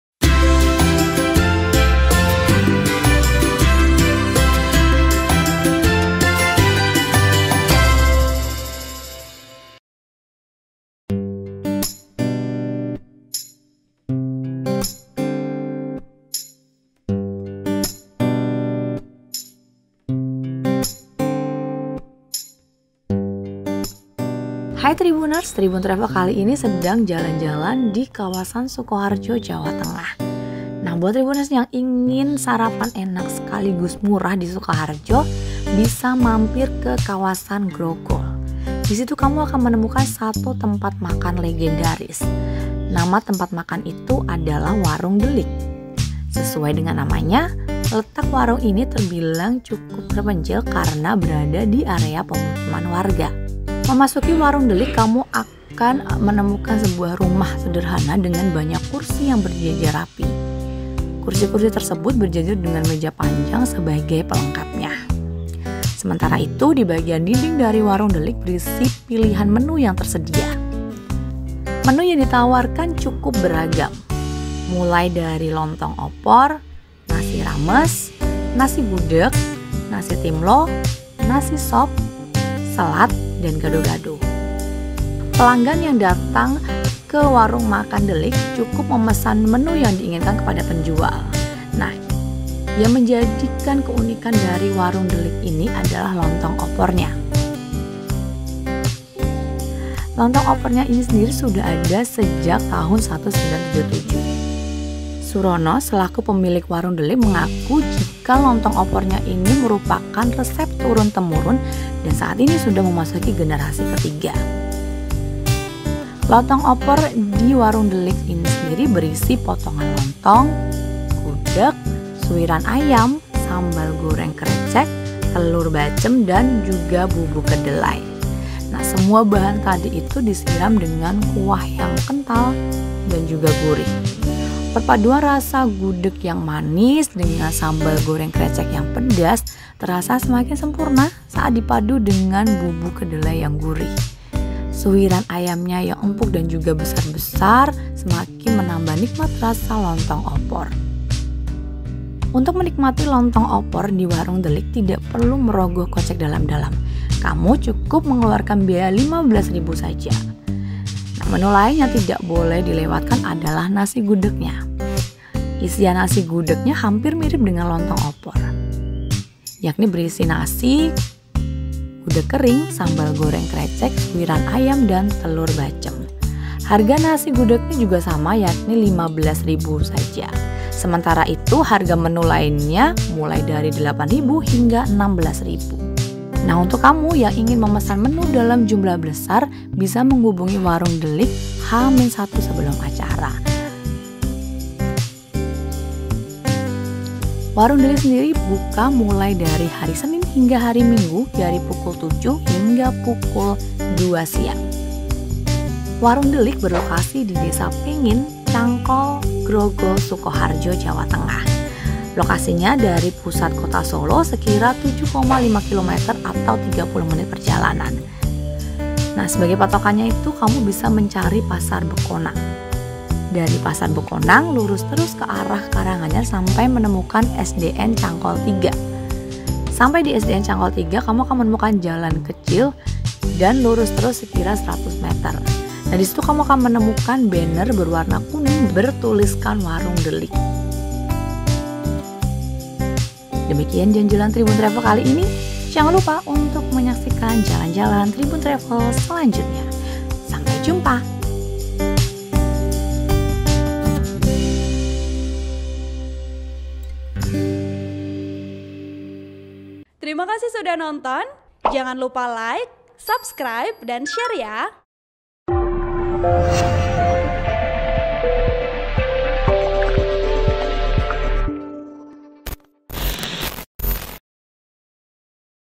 DIARY TRAVEL: Sarapan Lontong Opor di Warung Ndelik, Kuliner Legendaris di Sukoharjo
TRIBUN-TRAVEL.COM - Ingin sarapan enak dan murah di Sukoharjo Jawa Tengah?

Cobalah mampir ke kawasan Grogol Sukoharjo.

Di situ kamu akan menemukan satu tempat makan legendaris.
Nama tempat makan itu adalah Warung Ndelik.

Sesuai dengan namanya, letak warung ini cukup terpencil di area pemukiman warga.

Warung Ndelik berlokasi Desa Pengin, Cangkol, Sukoharjo Jawa Tengah.

Lokasinya dari pusat Kota Solo sekira 7,5 km atau 30 menit perjalanan.